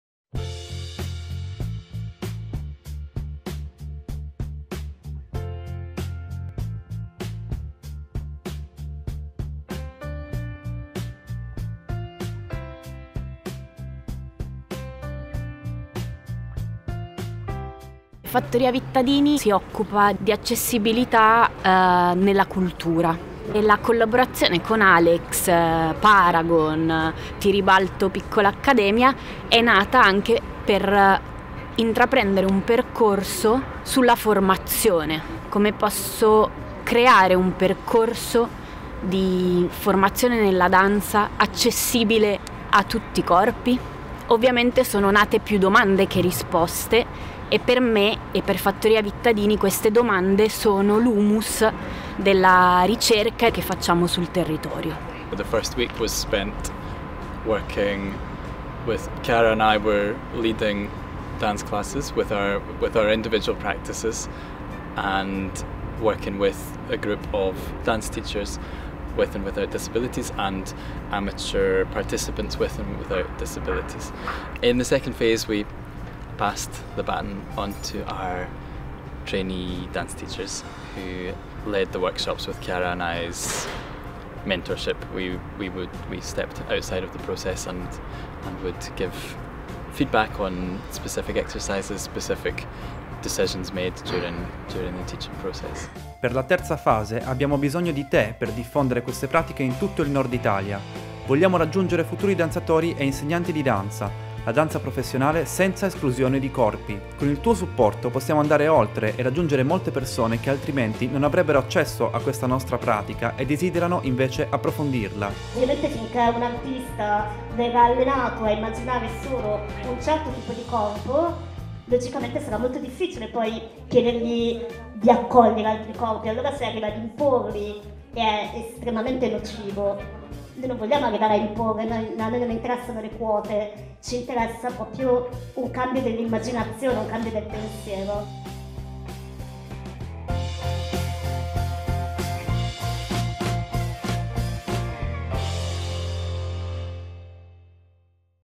S.I.I.A.T.E. (Scotland Italy Inclusive Artists Training Exchange)
A taster of our Inclusive Performing Artists training exchange with Fattoria Vittadini, in Milan.

Through the facilitation of training workshops and performance opportunities the ongoing project aims to broaden access to training, careers and experiences in dance throughout Italy and internationally.

SIIATE is supported the the British Council